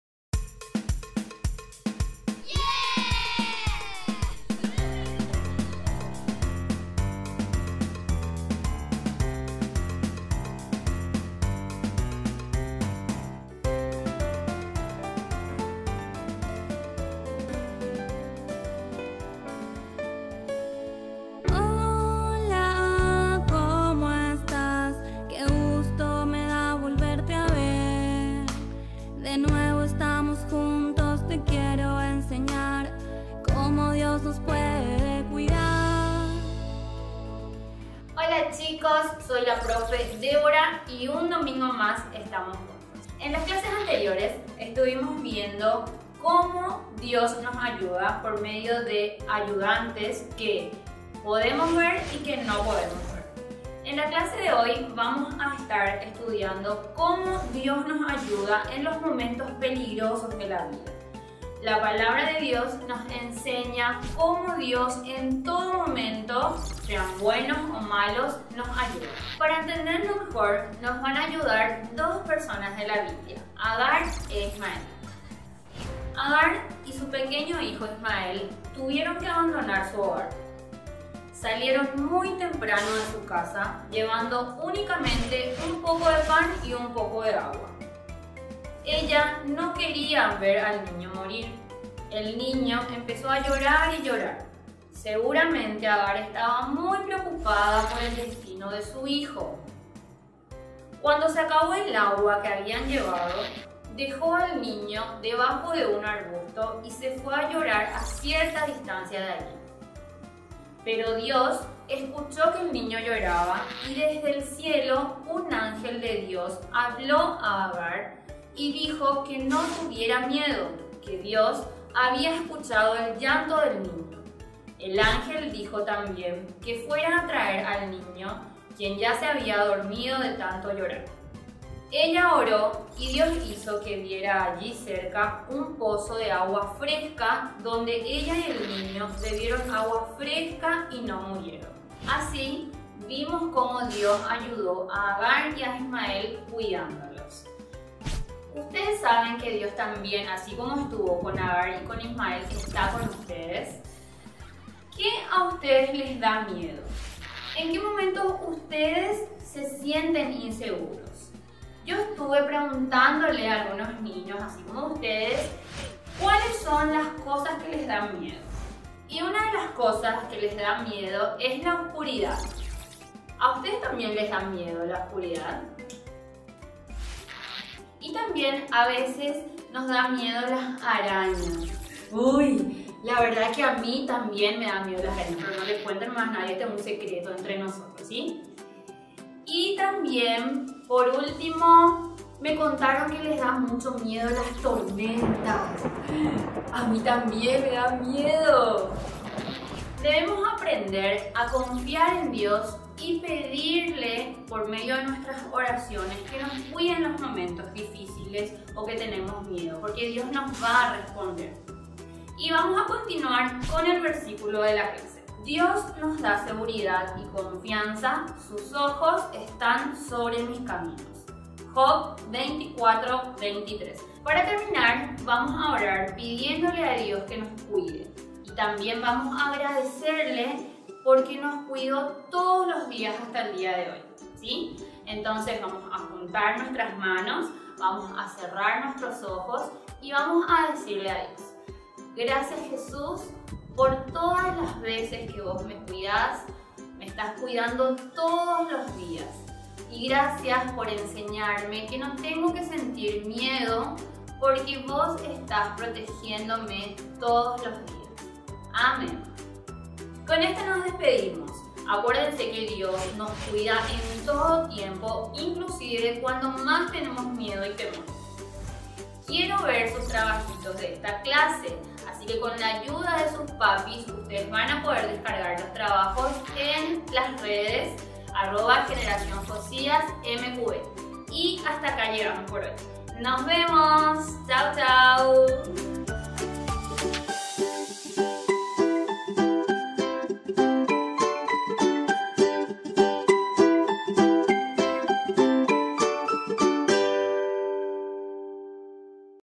MQV Niños - 3 a 5 años - El Cuidado de Dios - Lección 7
"Él les da seguridad…" 💪 (Job 24:23)

Hoy aprenderemos que Jesús siempre está listo para brindarnos seguridad por dondequiera que vayamos y que debemos tener confianza en pedir su ayuda en momentos de miedo. La Biblia nos muestra que Dios cuida de nosotros y nos da seguridad aún en los momentos peligrosos de la vida.

¡Esperamos ver tus trabajitos!😎
👉👉En este link vas a poder descargar la tarea 👇👇:

Podes dejarnos una foto 📸 de tu tarea o un video 🎬, debes publicarlo en tus redes con niñosmqvencasa"